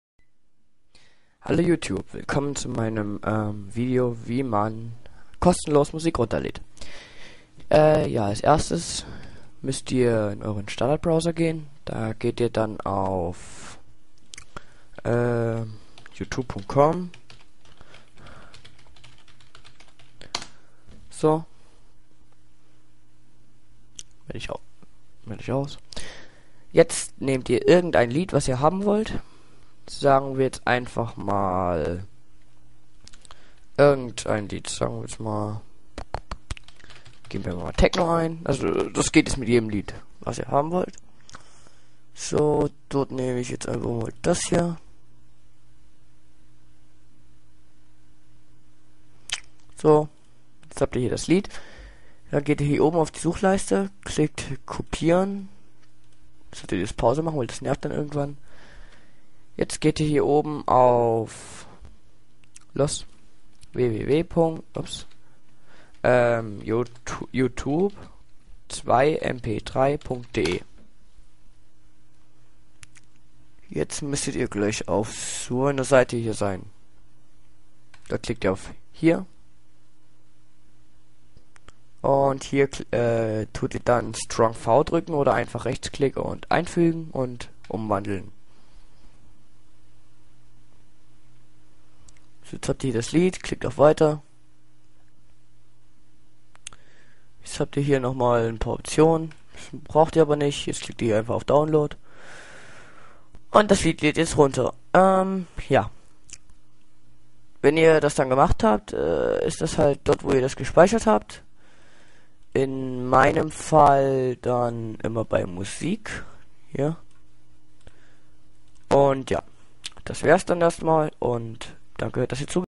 Kostenlos Musik downloaden [HD TUTORIAL]
In diesem Video zeige ich euch wie ihr euch gratis Musik downloaden könnt. Geht als erstes auf diesen Link und macht das selbe wie ich im Video.